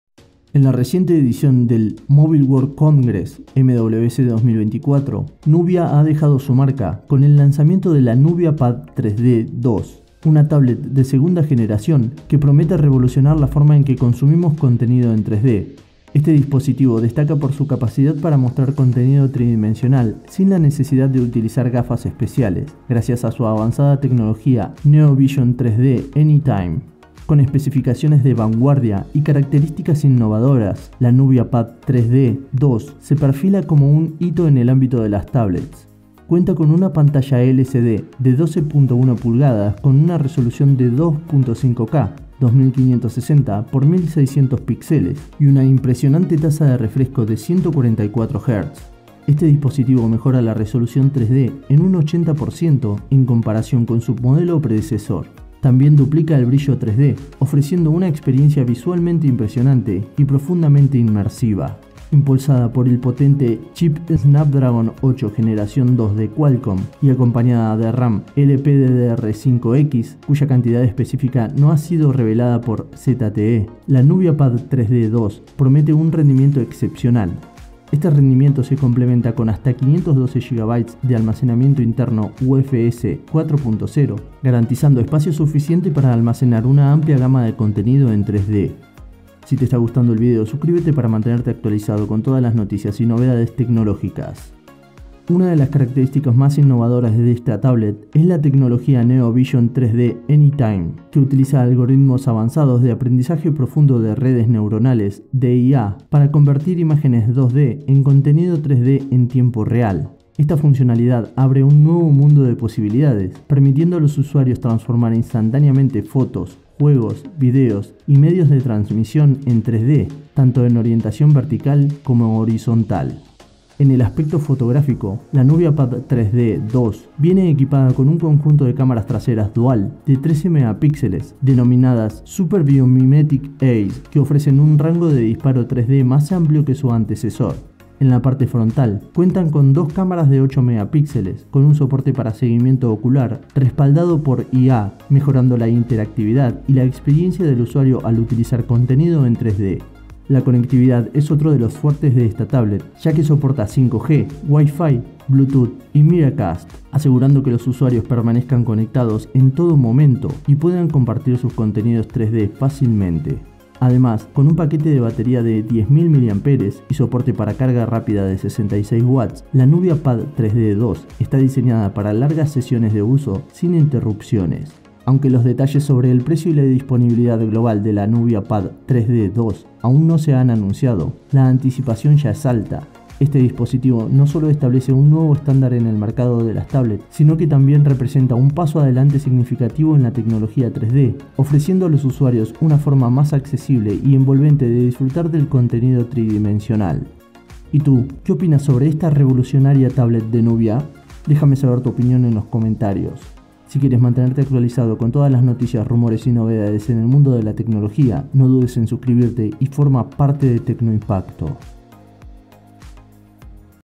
🚀¿El Fin de las Gafas 3D? Nubia Pad 3D II Impresiona y presenta su nueva tablet en MWC 2024
Descubre la innovación en 3D sin gafas con la Nubia Pad 3D II, presentada en el Mobile World Congress (MWC) 2024. Nubia redefine la experiencia multimedia con su última tablet, la Nubia Pad 3D II, que promete cambiar la forma en que disfrutamos del contenido en 3D. Gracias a su revolucionaria tecnología Neovision 3D Anytime, esta segunda generación de tablets permite visualizar contenido tridimensional sin necesidad de gafas especiales, estableciendo un nuevo hito en el ámbito de las tablets.

Con una pantalla LCD de 12.1 pulgadas que ofrece resolución 2.5K y una tasa de refresco de 144Hz, la Nubia Pad 3D II mejora significativamente la experiencia visual 3D, aumentando la resolución en un 80% y duplicando el brillo en comparación con su predecesor. Potenciada por el chip Snapdragon 8 Gen 2 de Qualcomm y equipada con RAM LPDDR5X y hasta 512GB de almacenamiento UFS 4.0, esta tablet no solo es un portento en rendimiento sino también un almacén para tu contenido 3D.

La tecnología Neovision 3D Anytime es una de las características más destacadas, capaz de transformar contenido 2D en 3D en tiempo real, abriendo un universo de posibilidades para fotos, juegos, videos y transmisiones. Sumado a esto, la Nubia Pad 3D II incorpora cámaras traseras duales de 13MP "Super Biomimetic Eye" y cámaras frontales duales de 8MP con seguimiento ocular por IA, enriqueciendo la interacción y experiencia de usuario con contenido en 3D.

La tablet también brilla en conectividad, soportando 5G, Wi-Fi, Bluetooth y Miracast, lo que asegura que permanezcas siempre conectado y puedas compartir fácilmente tu contenido 3D. Con una batería de 10,000mAh y carga rápida de 66W, la Nubia Pad 3D II está diseñada para largas horas de disfrute sin interrupciones.

Mantente al tanto de las últimas noticias sobre la disponibilidad global y el precio de la Nubia Pad 3D II, un dispositivo que no solo eleva el estándar en el mercado de las tablets sino que también representa un avance significativo en la tecnología 3D.